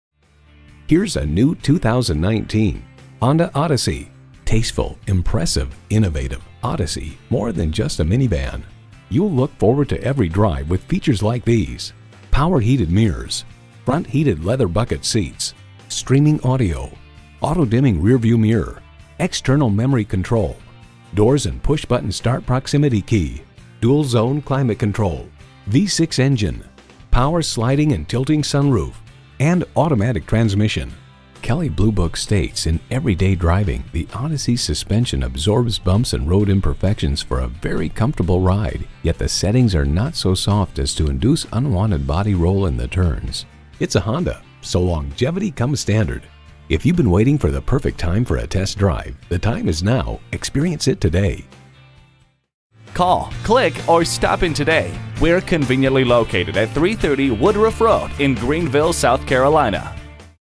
New 2019 Honda Odyssey Greenville SC Easley, SC #191484 - SOLD
Year: 2019
Make: Honda
Model: Odyssey
Trim: EX-L
Engine: 3.5L V6 SOHC i-VTEC 24V
Transmission: 9-Speed Automatic
Color: White
Interior: Bge Lth
Stock #: 191484
VIN: 5FNRL6H76KB093050

Address:
330 Woodruff Road